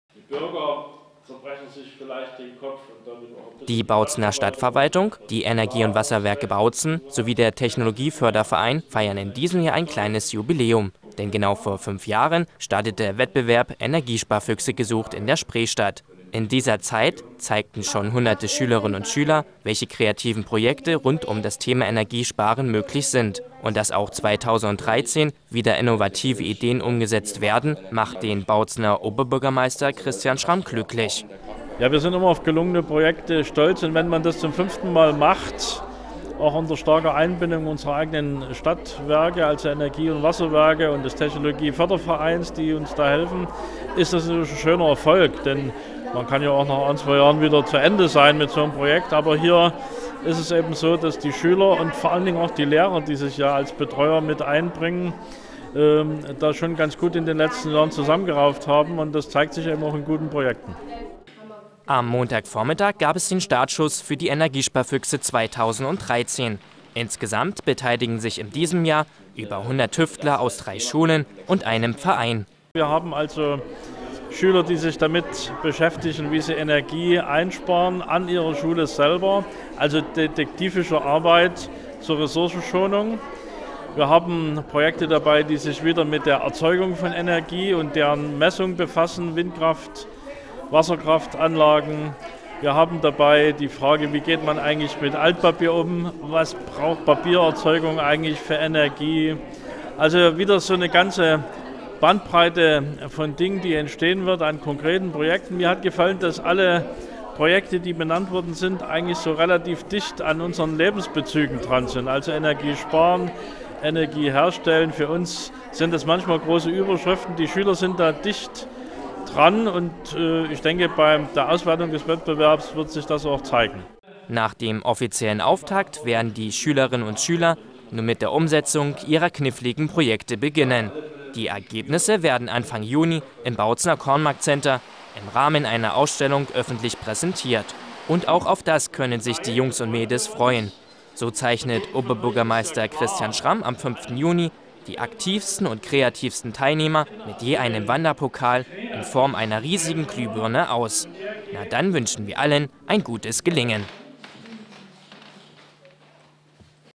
Auftakt Energiesparfüchse 2013
Die Bautzener Stadtverwaltung, die Energie- und Wasserwerke Bautzen sowie der Technologieförderverein feiern in diesem Jahr ein kleines Jubiläum. Denn genau vor 5 Jahren startete der Wettbewerb „Energiesparfüchse gesucht" in der Spreestadt.

In dieser Zeit zeigten schon hunderte Schülerinnen und Schüler, welche kreativen Projekte rund um das Thema Energiesparen möglich sind. Und das auch 2013 wieder innovative Ideen umgesetzt werden, macht den Bautzener Oberbürgermeister Christian Schramm glücklich.

Am Montagvormittag gab es den Startschuss für die Energiesparfüchse 2013. Insgesamt beteiligen sich in diesem Jahr über 100 Tüftler aus drei Schulen und einem Verein. Nach dem offiziellen Auftakt werden die Schülerinnen und Schüler nun mit der Umsetzung ihrer kniffligen Projekte beginnen.

Die Ergebnisse werden Anfang Juni im Bautzener Kornmarkt-Center im Rahmen einer Ausstellung öffentlich präsentiert. Und auch auf das können sich die Jungs und Mädels freuen: So zeichnet Oberbürgermeister Christian Schramm am 5. Juni die aktivsten und kreativsten Teilnehmer mit je einem Wanderpokal in Form einer riesigen Glühbirne aus.

Na dann wünschen wir allen ein gutes Gelingen!.